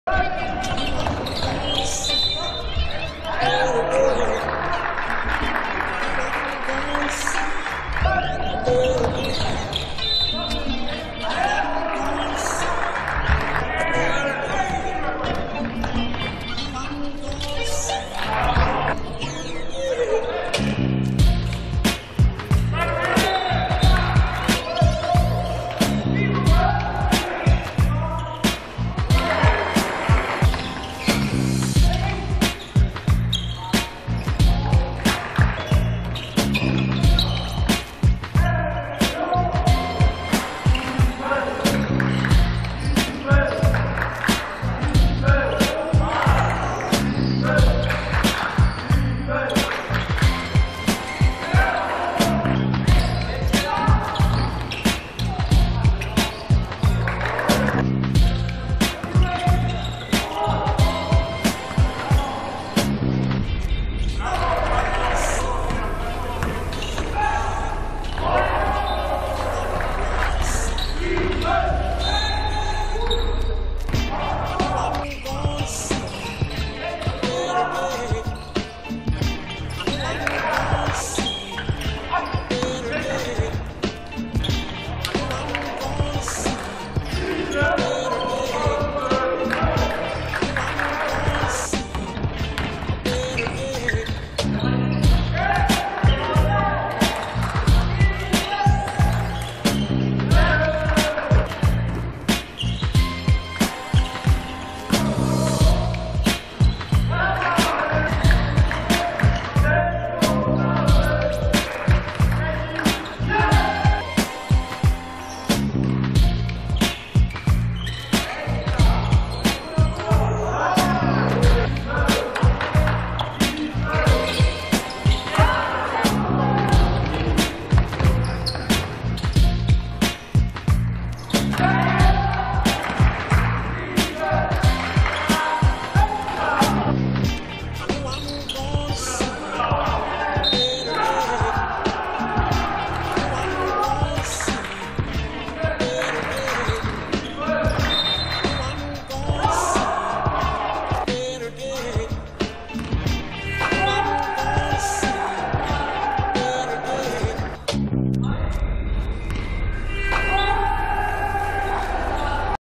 25 07 Boston vs Oklahoma – Draft Camp 13
BOSTON 65:57 OKLAHOMA
A.Lubowiedzki 23pkt 22zb
D.Mielczarek 17pkt 13zb